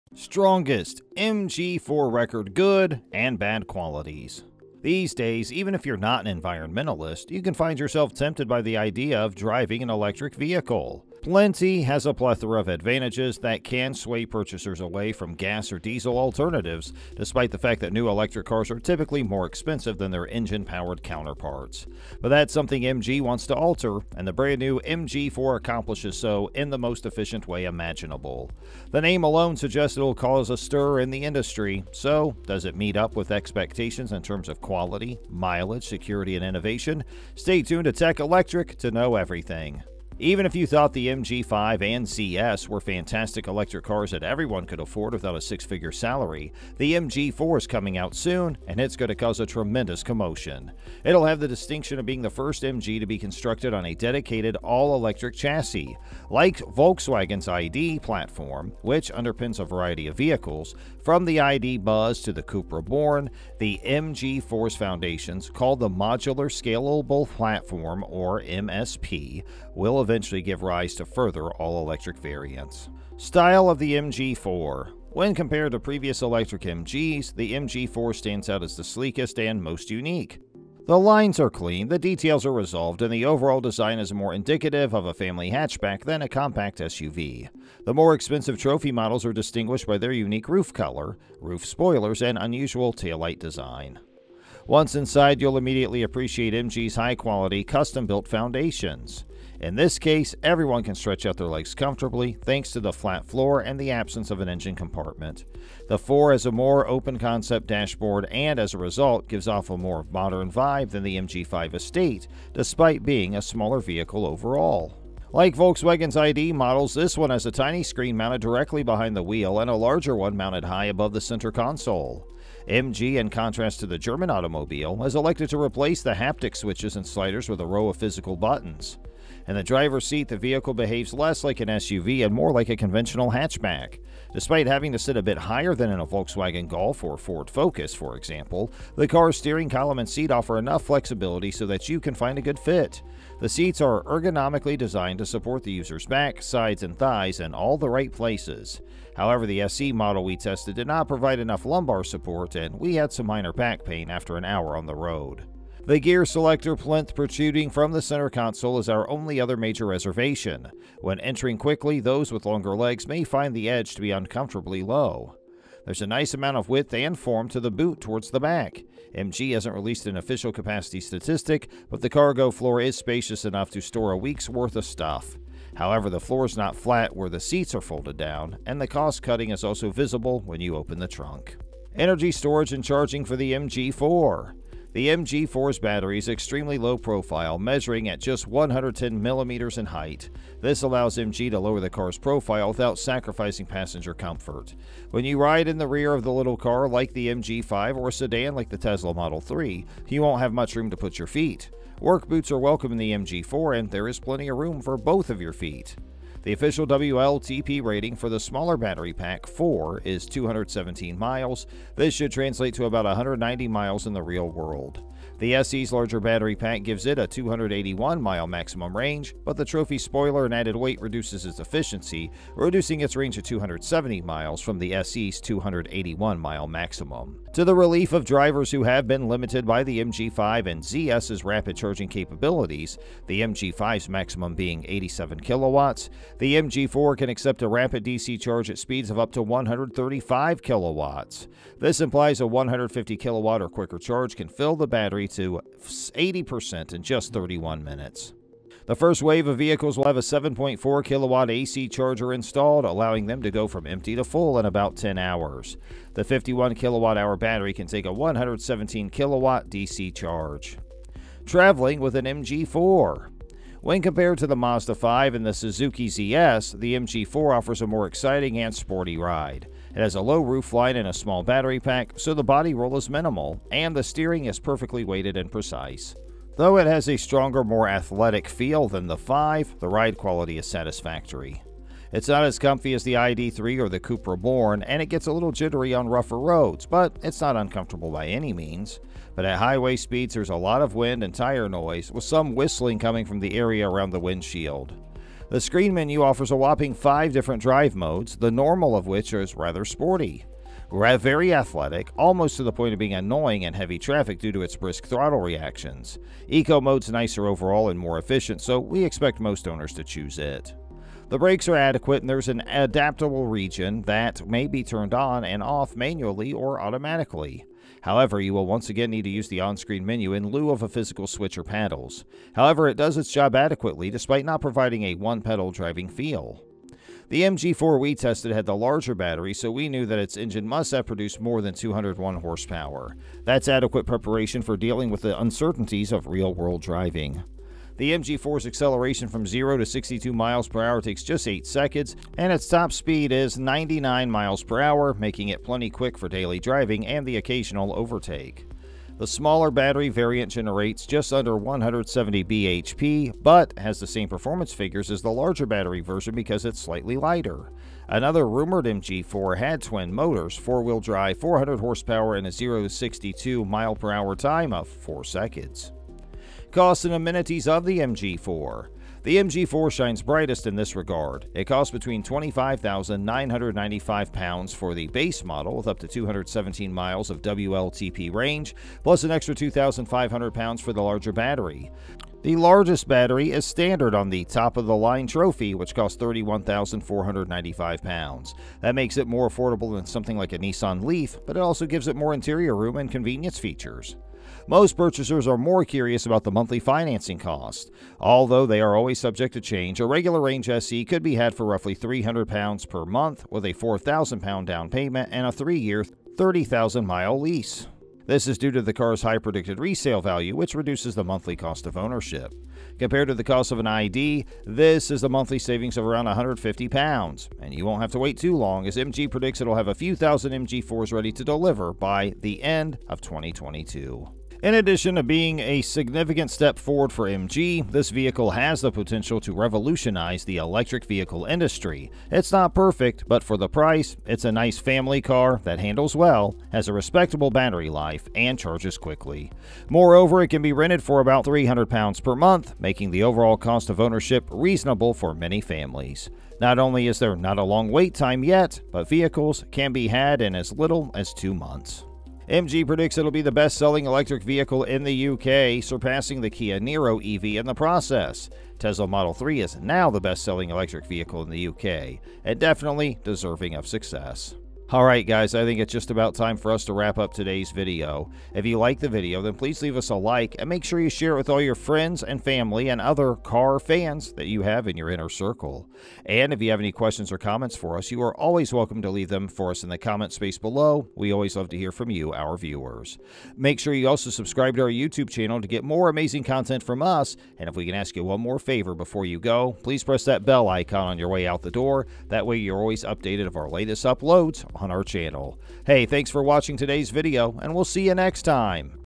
STRONGEST MG4 RECORD GOOD and BAD Qualities
These days, even if you're not an environmentalist, you can find yourself tempted by the idea of driving an electric vehicle. Plenty has a plethora of advantages that can sway purchasers away from petrol or diesel alternatives, despite the fact that new electric cars are typically more expensive than their engine-powered counterparts. But that's something MG wants to alter, and the brand-new MG4 accomplishes so in the most efficient way imaginable. The name alone suggests it will cause a stir in the industry, but does it meet up to expectations in terms of quality, mileage, security, and innovation?

Even if you thought the MG 5 and ZS were fantastic electric cars that everyone could afford without a six-figure salary, the MG4 is coming out soon, and it's going to cause a tremendous commotion. It will have the distinction of being the first MG to be constructed on a dedicated, all-electric chassis. Like Volkswagen's ID. platform, which underpins a variety of vehicles, from the ID. Buzz to the Cupra Born, the MG4's foundations (called the Modular Scalable Platform (MSP)) will eventually give rise to further all-electric variants.

About Tech Electric
🎥 Videos about Electric Vehicles (EV), EV Batteries, Nuclear Diamond Battery and more
🎨 Written, voiced and produced by Tech Electric